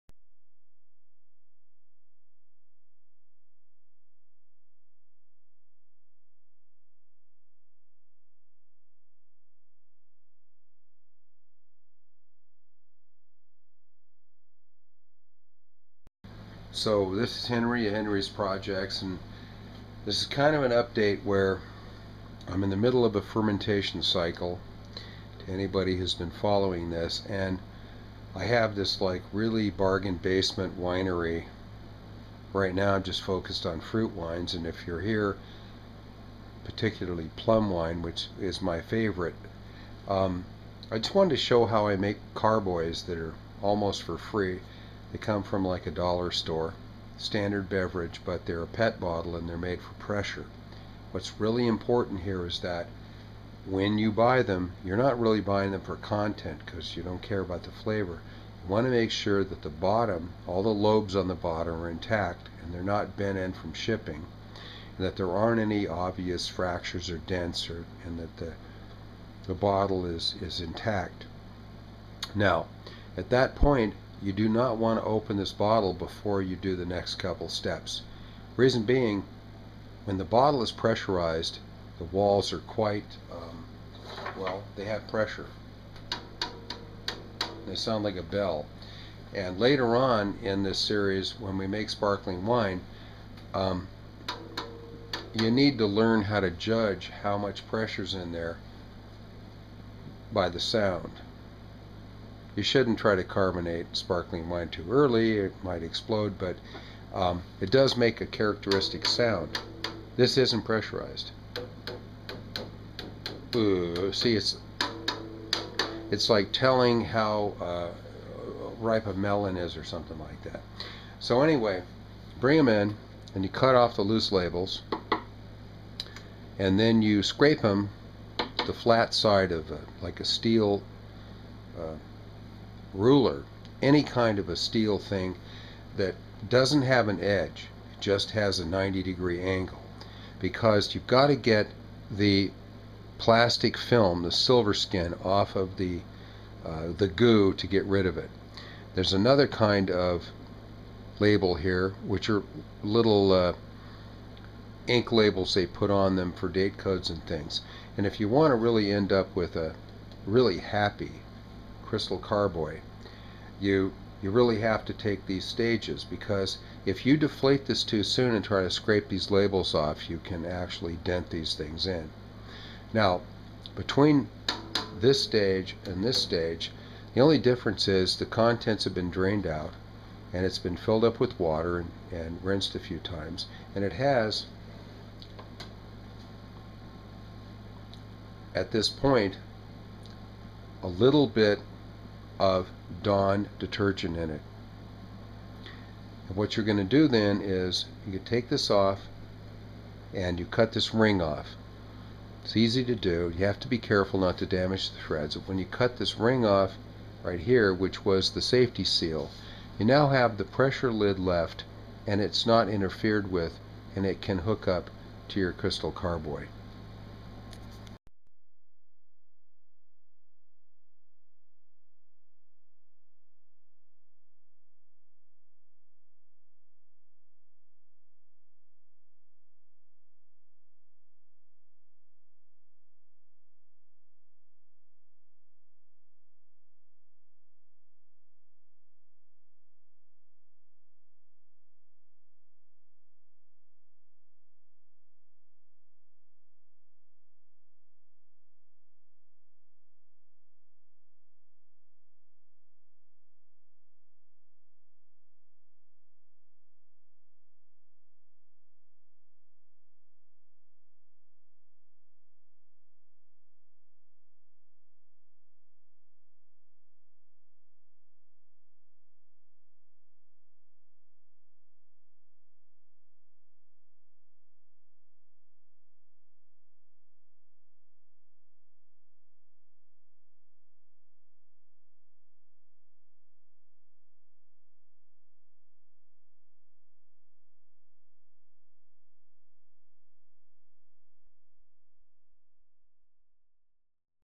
making plum wine part 3 no music
This segment of making Plum Wine is about the fermentation vessels. These are known as carboys. Using PET Soda Bottles is a real easy way to ferment. Instead of fermenting 5 gallons at 40 pounds each the PET Carboy weighs just 4 pounds. Moving a full bottle is no problem. If a batch isn't right the loss is minimal.